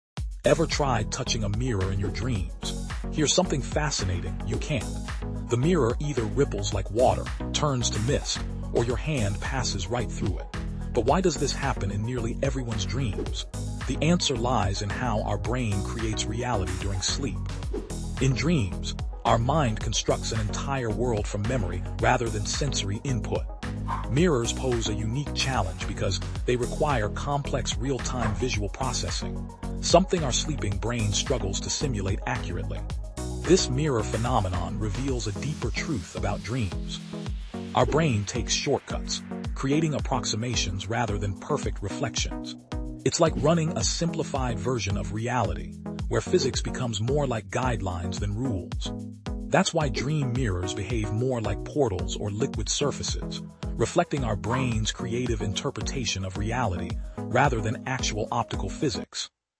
Why Can’t We Touch Mirrors in Dreams?
Ever wondered why you can't touch mirrors in your dreams? Discover the fascinating science behind dream mirrors and how our brain constructs reality during sleep. Learn why mirrors appear as rippling water, mist, or portals, revealing insights into how dreams work.